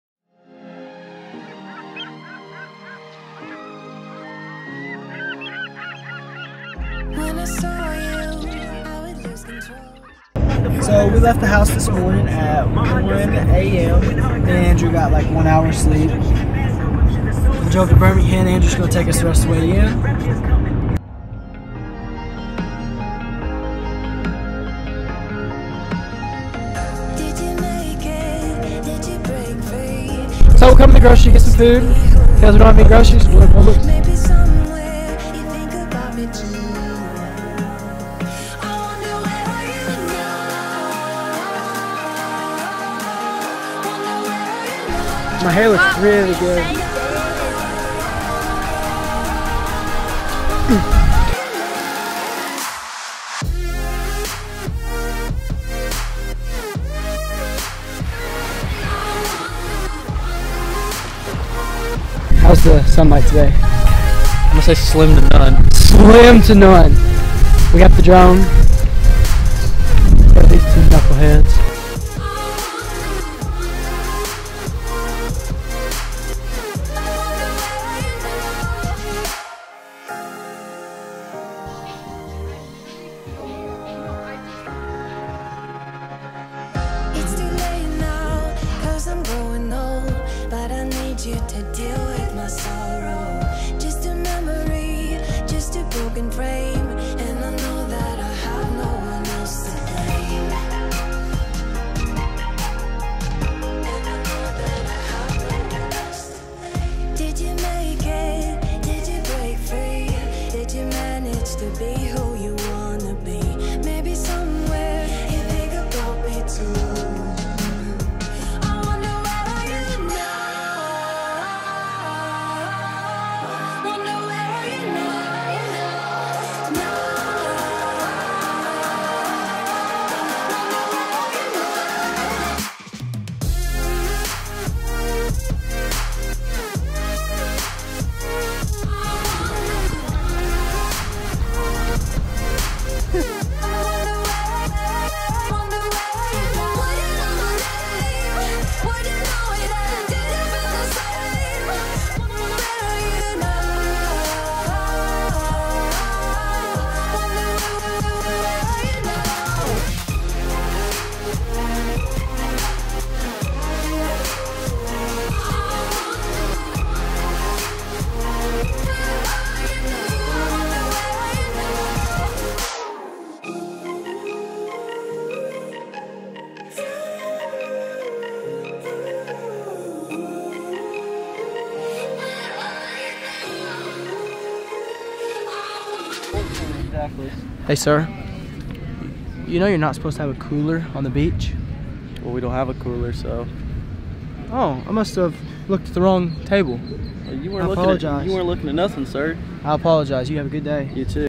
4 Beaches in 4 Days
From Panama City beach, to fort Meyers, to Fort Lauderdale, and Sanibel Island